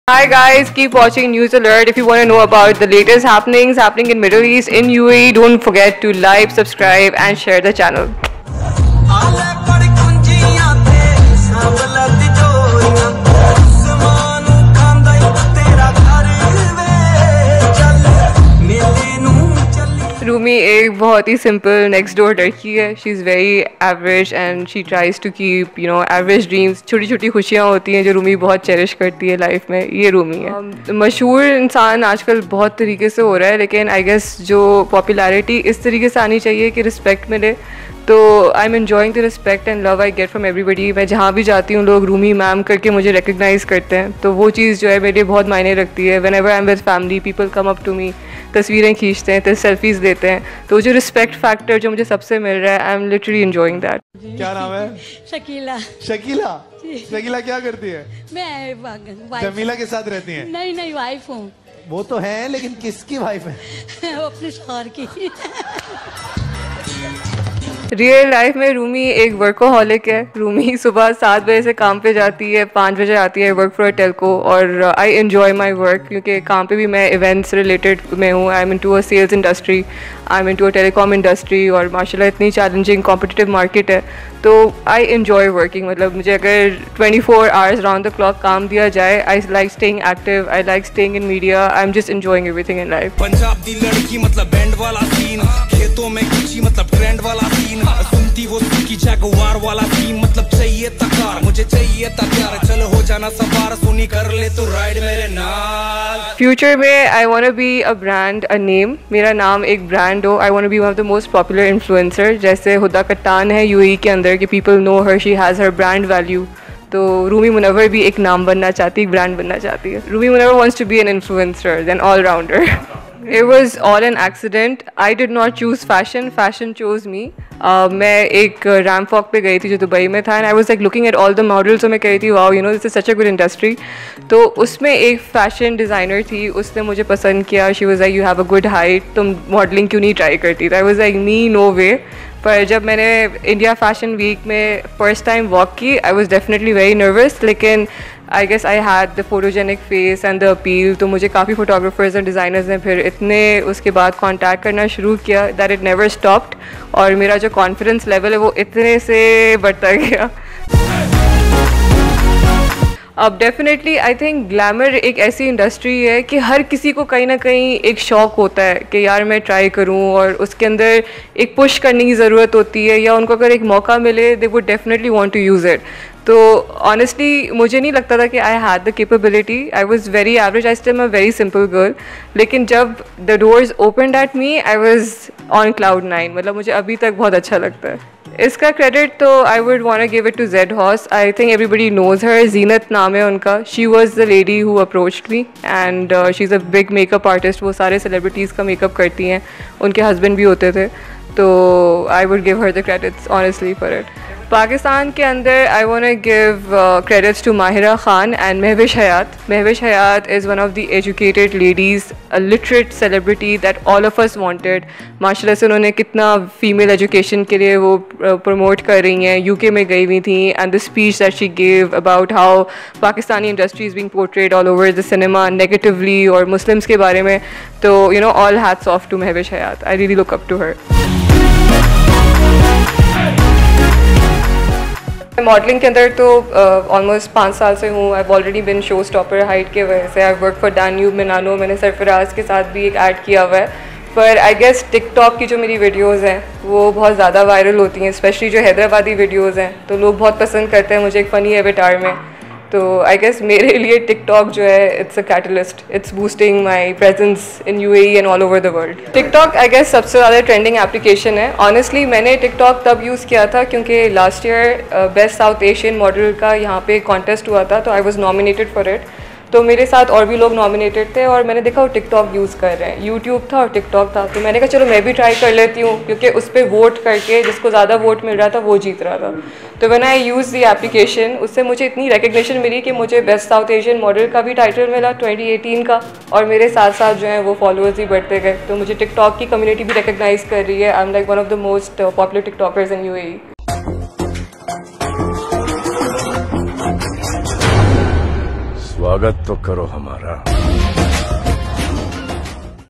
Tik Tok Sensation Rumi Munawar's Exclusive Interview | Most famous tik tok star | NewsAlert | 2020
سوشل میڈیا سے شہرت کی بلندیوں کو چھو لینے والی ماڈل اور ٹک ٹاک سٹار،رومی منور کی کہانی ، ان کی زبانی